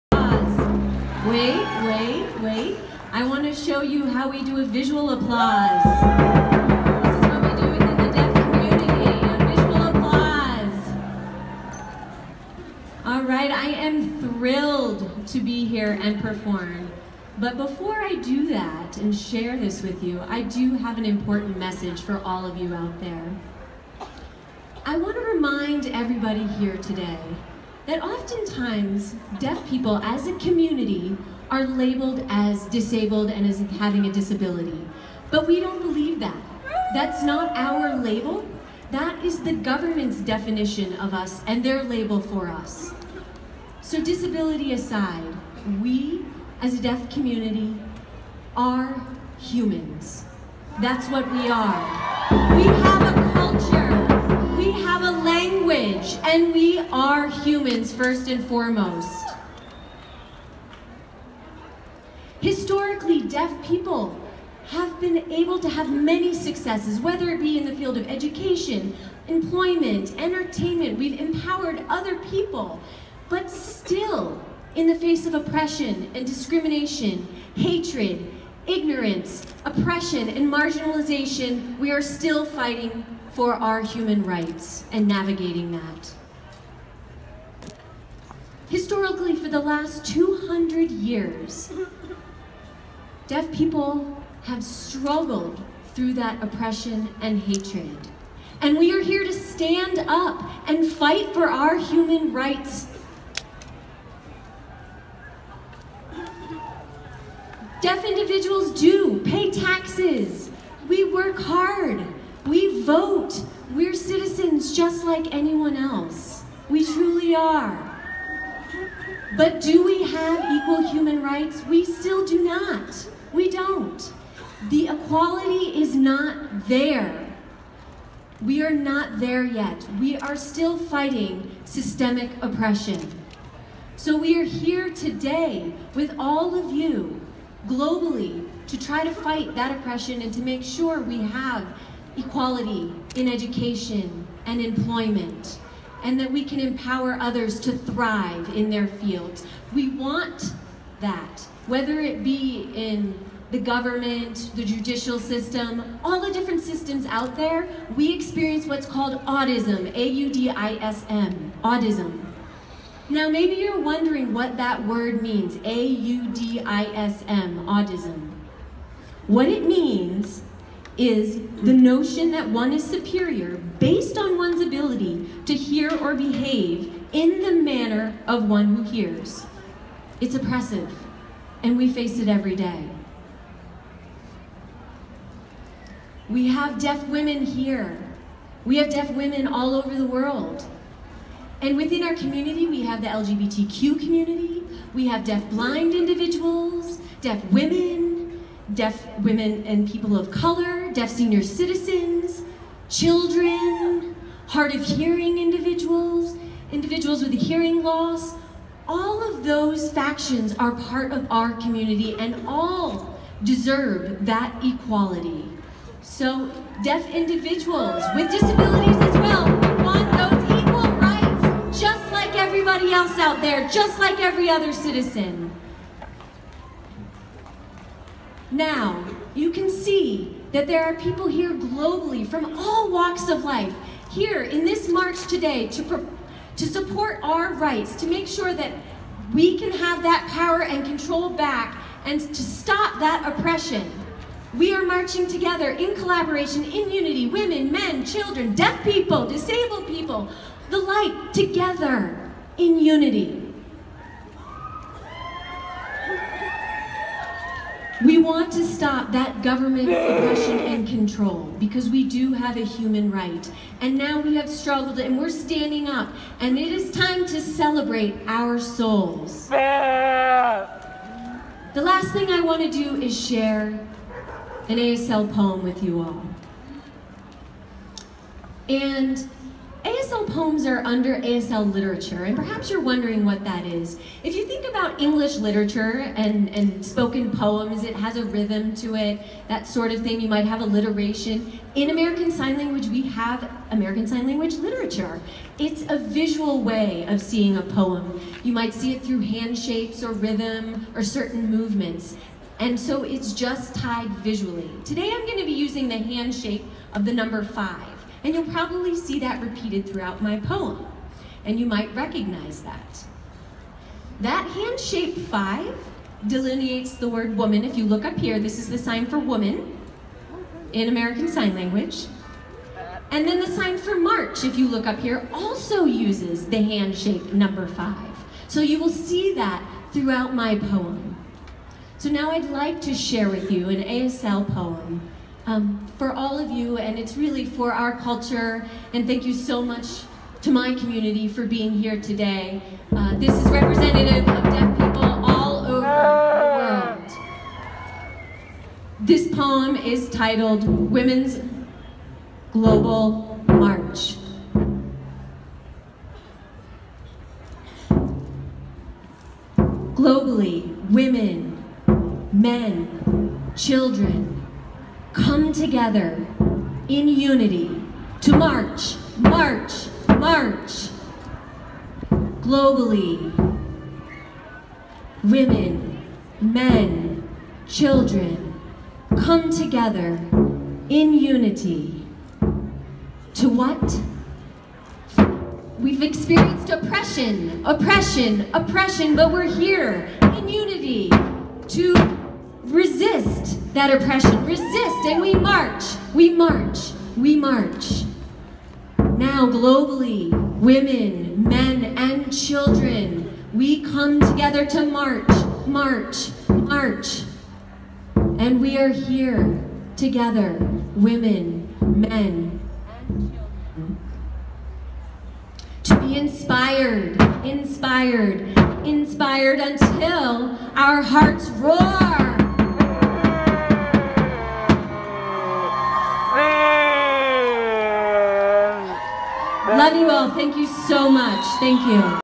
Women March on Denver - ASL Poetry
Ruthie gave a presentation before sharing her ASL poetry about Deaf human rights at the Women March on Denver on January 21, 2017.
Almost 200,000 people marched.

Becky Wells - Voice Interpreter
Shawn Goodman Miller - Voice Interpreter